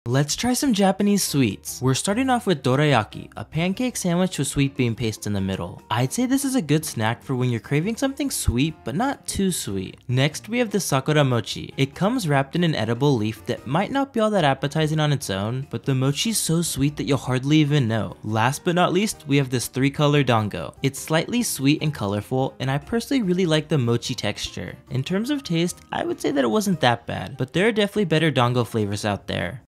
Let’s Try Some Japanese Sweets
In this video, we will try 3 Japanese sweets.